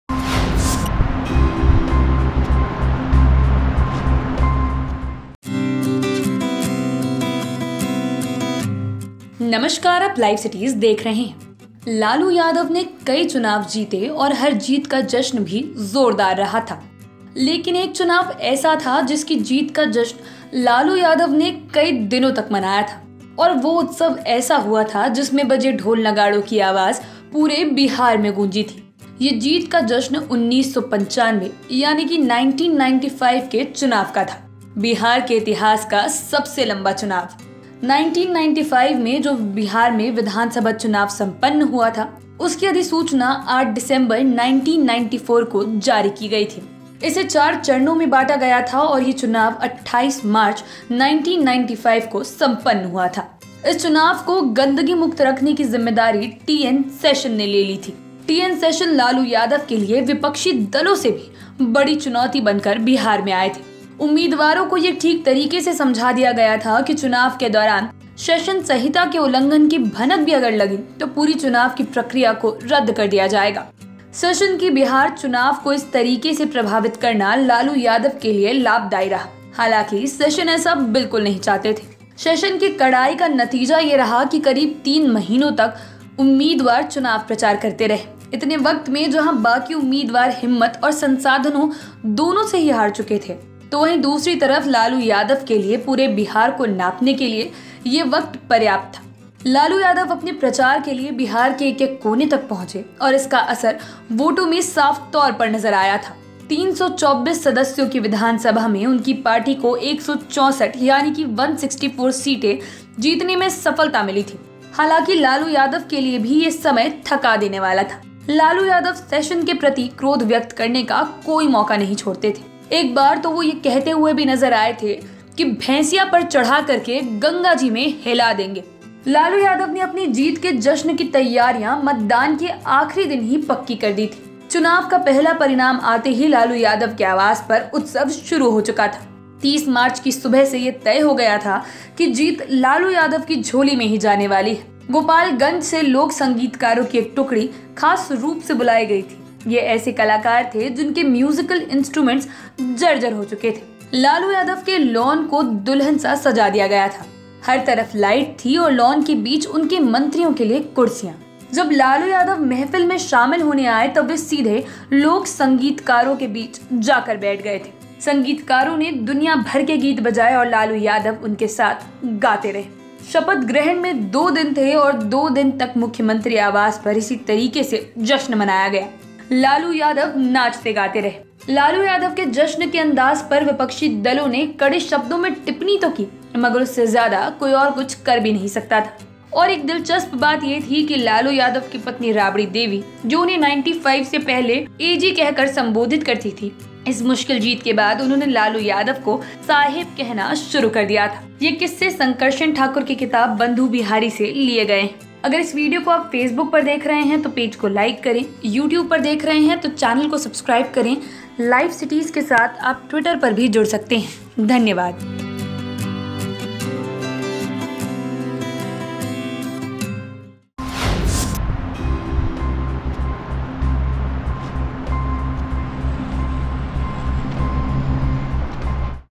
Lalu Yadav के लिए ये थे सबसे बड़ा सिर दर्द, तीन महीने तक Bihar में दौड़ाया था, फिर क्या हुआ, देखिए
Lalu Yadav के लिए ये थे सबसे बड़ा सिर दर्द, तीन महीने तक Bihar में दौड़ाया था, फिर क्या हुआ, देखिए | Bihar News | LiveCities